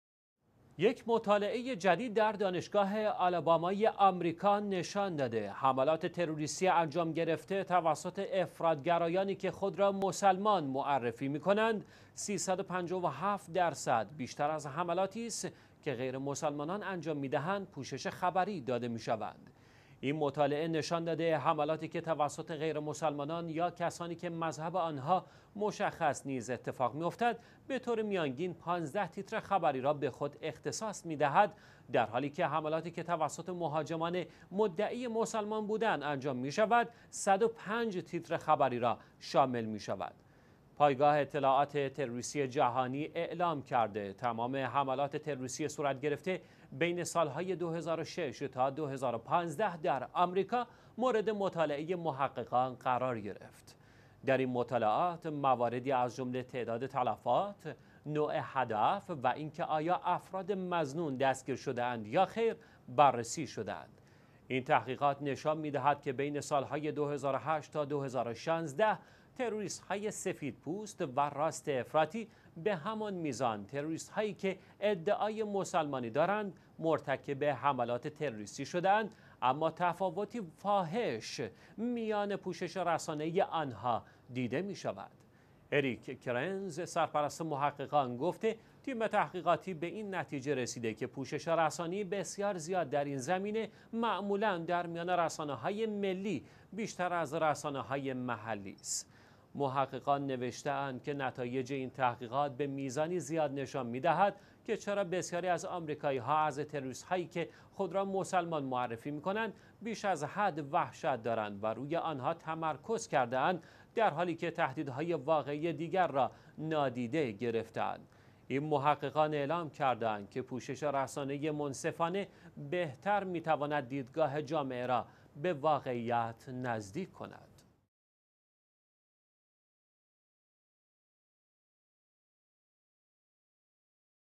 پوشش رسانه ای حملات مهاجمان مدعی اسلام، ۳۵۷ درصد بیشتر از حملات غیرمسلمانان، در آمریکا
یک مطالعه در آمریکا نشان داد که حملات تروریستی انجام گرفته توسط افراط گرایانی که خود را مسلمان معرفی می کنند 357 درصد بیشتر از حملاتی پوشش خبری داده می شوند که عناصر آنها ادعای اسلام ندارند.
یک مطالعه جدید توسط دانشگاه آلابامای آمریکا افشاء کرد که حملات تروریستی انجام گرفته توسط سنی های تندرو که ادعای مسلمان بودن دارند، 357 درصد بیش از حملاتی که عاملان آنها غیر مسلمان هستند توسط خبرگزاری ها پوشش داده می شوند.
این مطالعه نشان داد، حملاتی که توسط غیرمسلمانان یا کسانی که مذهب آن ها مشخص نیست اتفاق افتاده، به طور میانگین 15 تیتر خبری را به خود اخصاص می دهند، درحالی که حملات مهاجمانی که مدعی مسلمان بودن را دارند، 105 تیتر خبری را شامل می شوند.
با توجه به پایگاه اطلاعات تروریستی جهانی، محققان تمام حملات تروریستی در آمریکا بین سال های 2006 تا 2015 را مورد مطالعه قرار دادند.
در این مطالعات عواملی چون تعداد تلفات، نوع هدف و اینکه آیا افراد مظنون دستگیر شده اند یا خیر کنترل گردیده است.

برای ادامه خبر به سایت خبرگزاری شیعه ویوز مراجعه فرمائید...